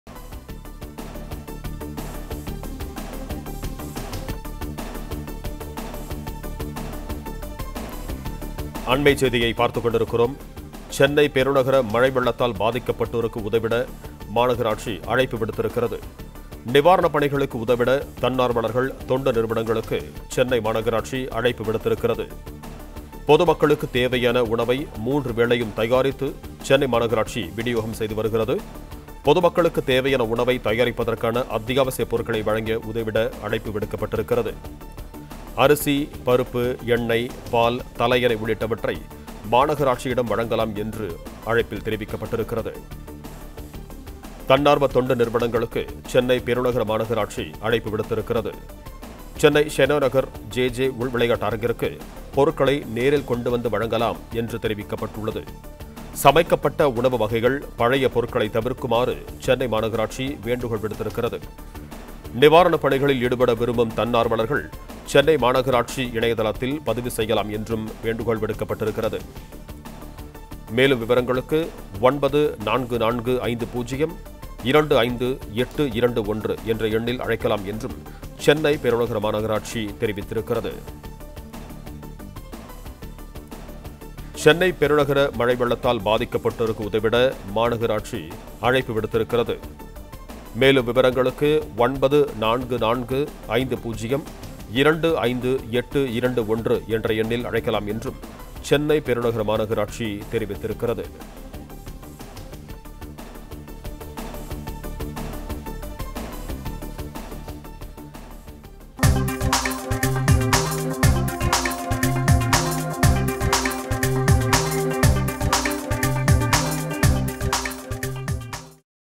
தன்னார்வ தொண்டு நிறுவனங்களுக்கு அழைப்பு..!
சென்னை பெருநகர மழை, வெள்ளத்தால் பாதிக்கப்பட்டோருக்கு உதவிட மாநகராட்சி அழைப்பு

நிவாரணப் பணிகளுக்கு உதவிட, தன்னார்வலர்கள், தொண்டு நிறுவனங்களுக்கு சென்னை மாநகராட்சி அழைப்பு